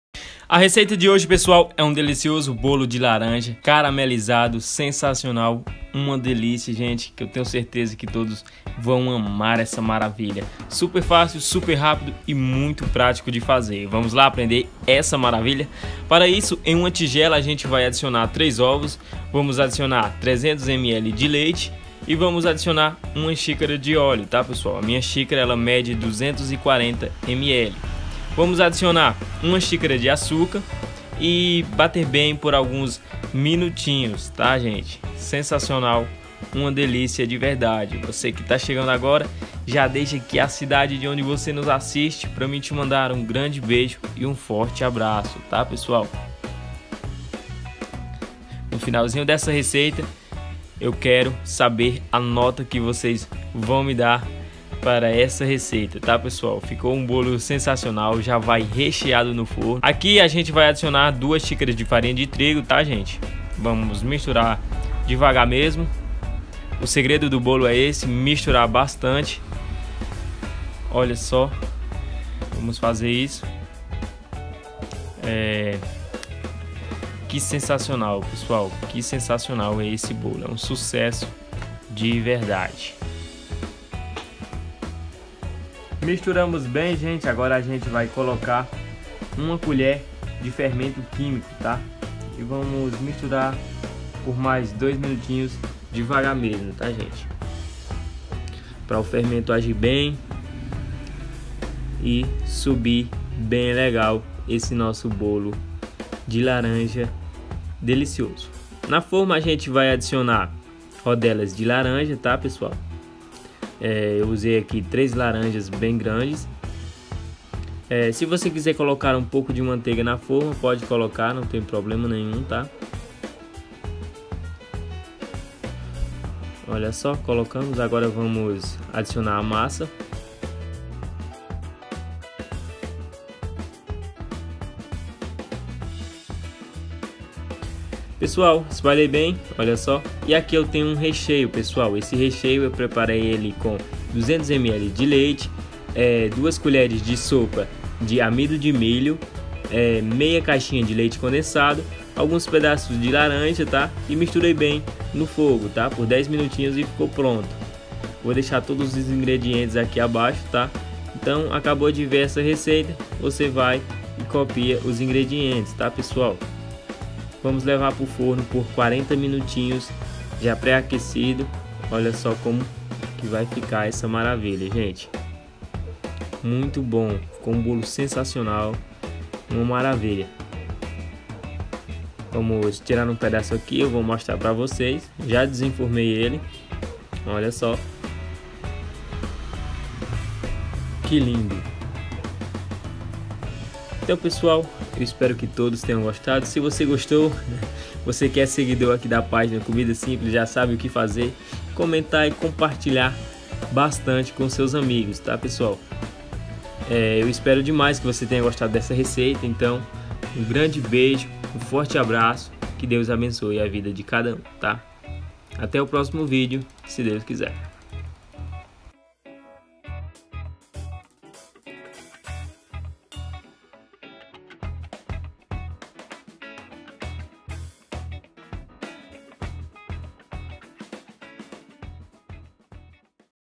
Bolo de laranja caramelado! Não precisa fazer o caramelo é so mexer super fácil
BOLO DE LARANJA
3 LARANJAS GRANDES
3 OVOS
300 ML DE LEITE
1 X DE ÓLEO
1 X DE AÇÚCAR
2 X DE FARINHA DE TRIGO
1 COLHER [SOPA] DE FERMENTO QUÍMICO
RECHEIO:
200 ML DE LEITE
2 COLHERES [SOPA] DE AMIDO DE MILHO
1/2 CX DE LEITE CONDENSADO
PEDAÇOS DE LARANJA
FORNO 40 MINUTOS
COMENTA AQUI A CIDADE DE ONDE VOCE NOS ASSISTE TA?
MEU ENDEREÇO:
Rua Iguatu
N° 83
Bairro: São Vicente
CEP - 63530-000
Ceará - Cariús
Anunciado Cleber Monteiro